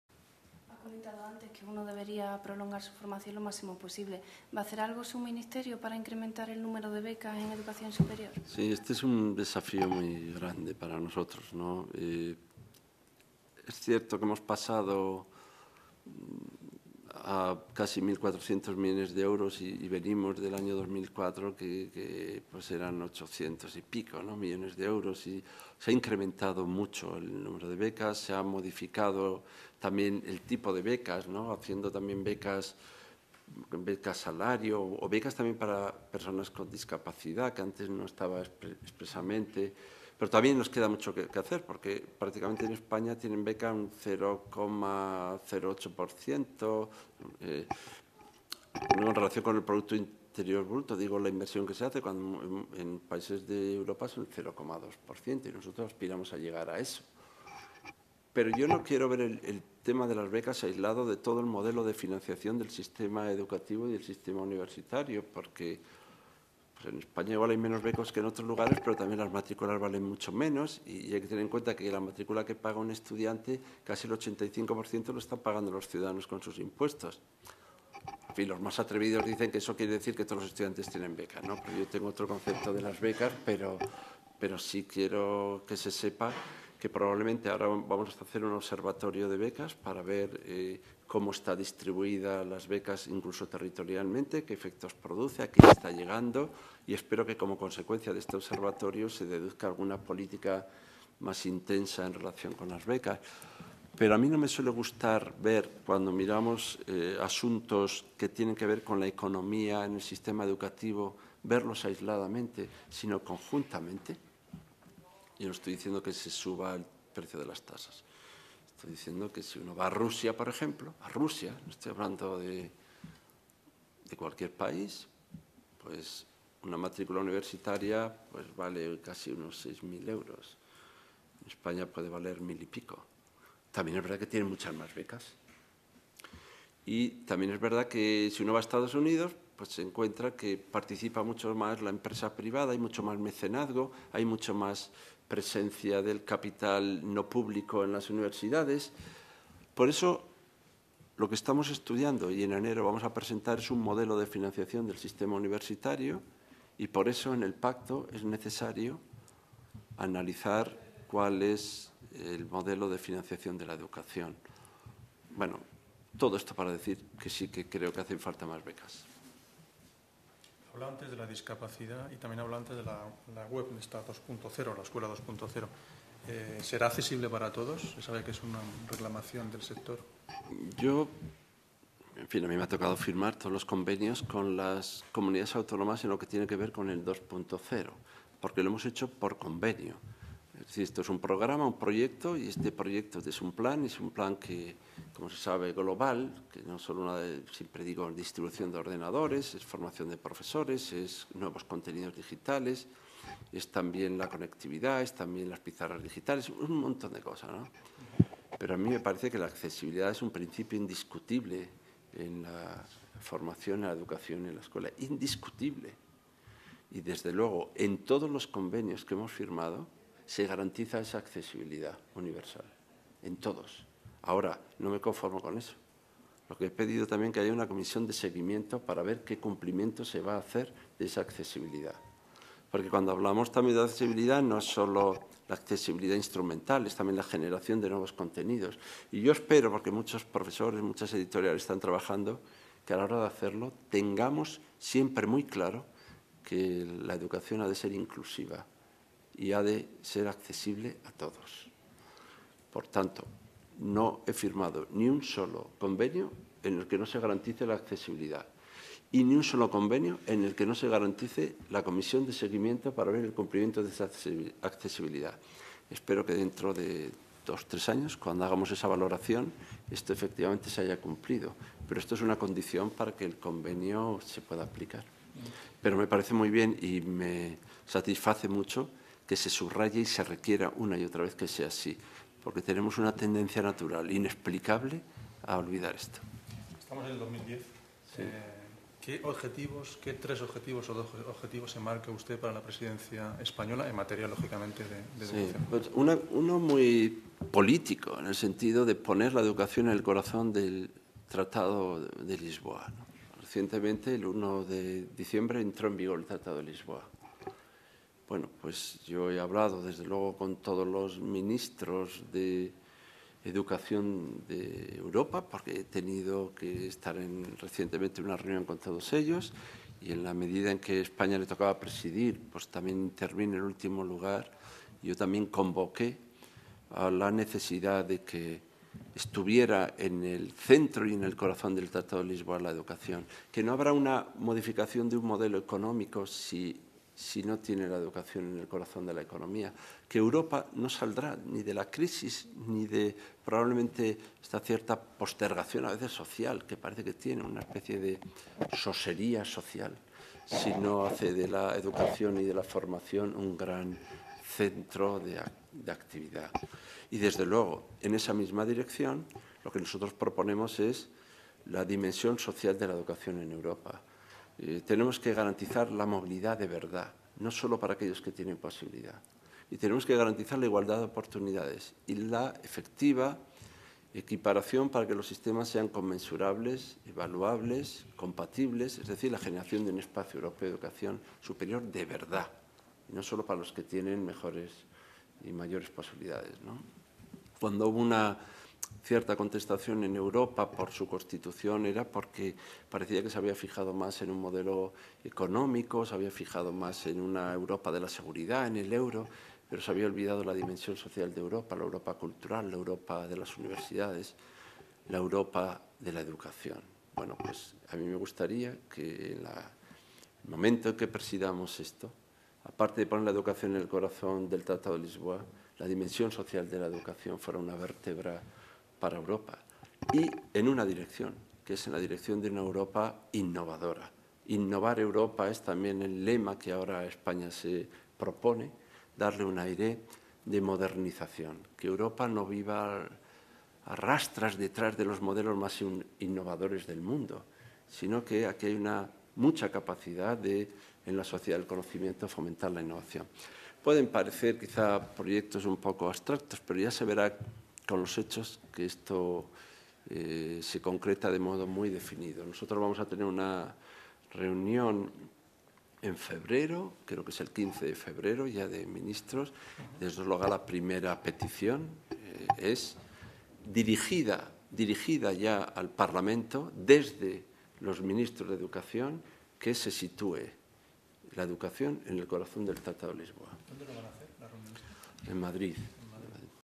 4ª Parte- Entrevista a Ángel Gabilondo
Entrevista de la agencia Servimedia con el Ministro de Educación, Ángel Gabilondo. Cuarta Parte